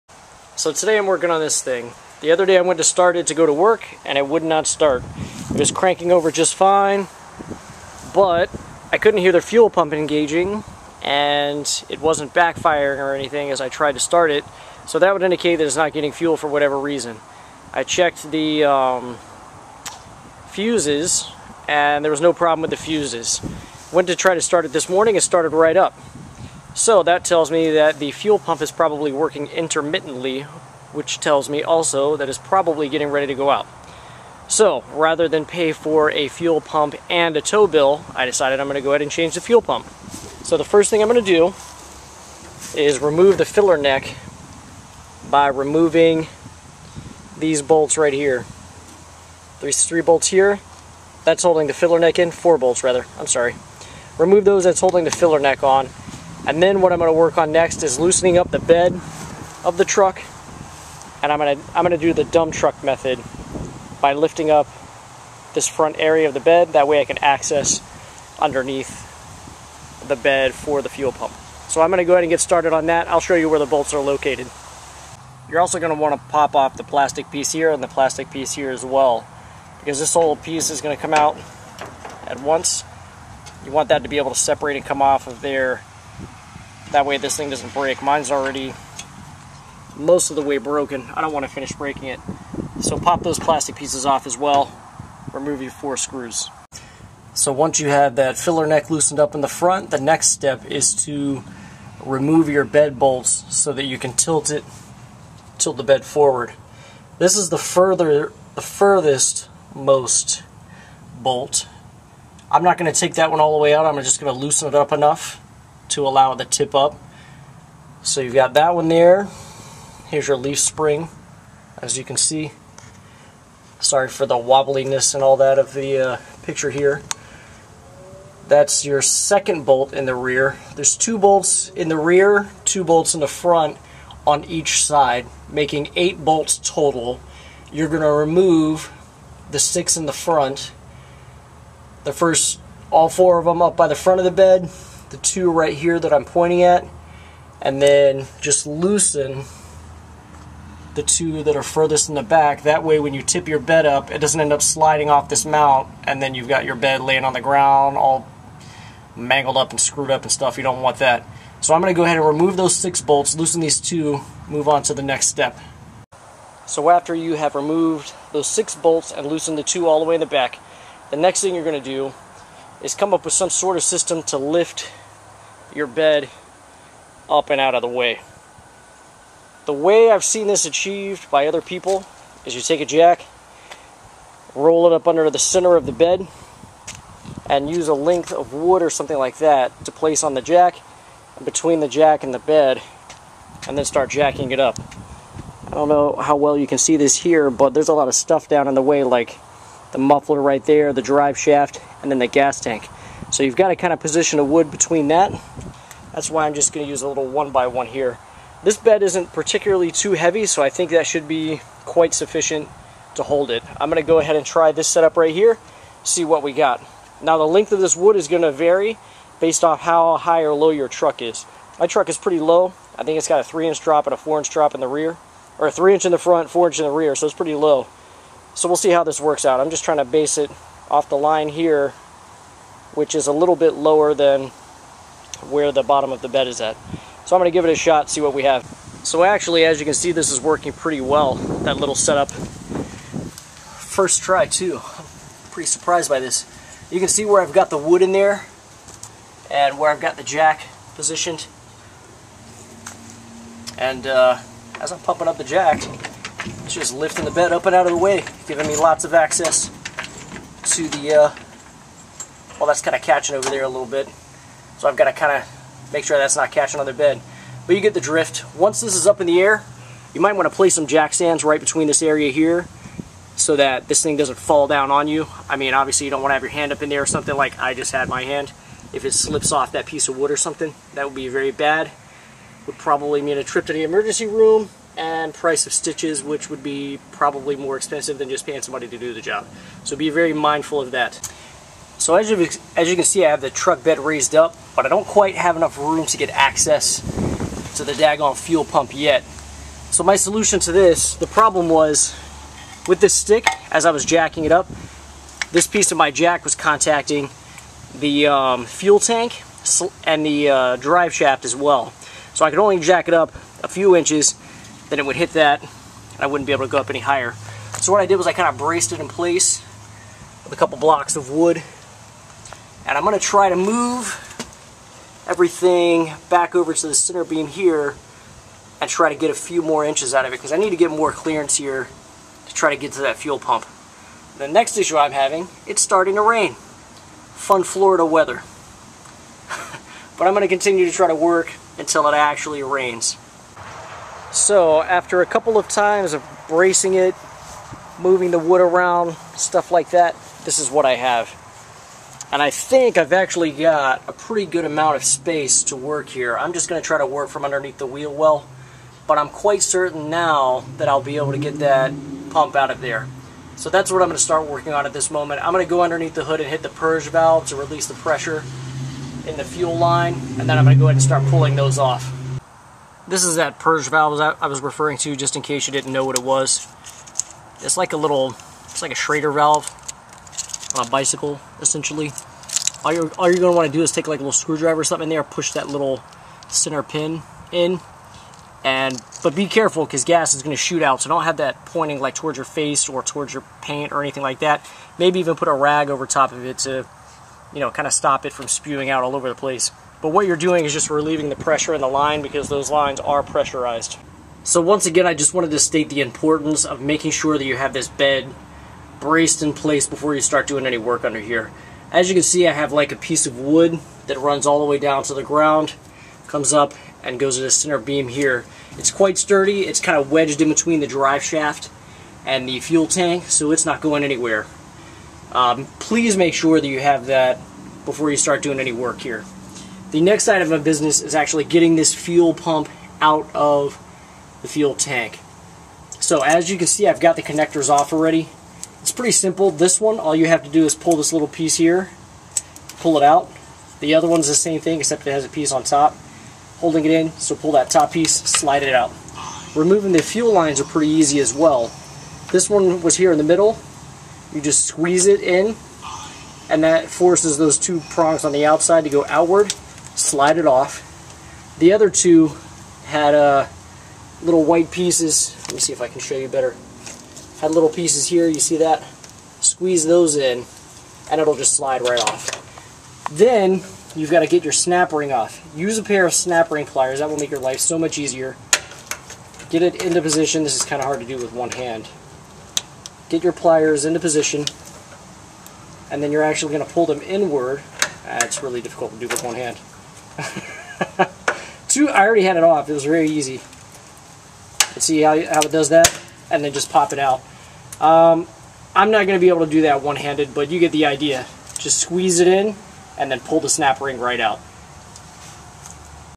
S10 Fuel Pump Replacement Part 1
Me replacing a fuel pump on my 98 S10, by using the tilting bed method.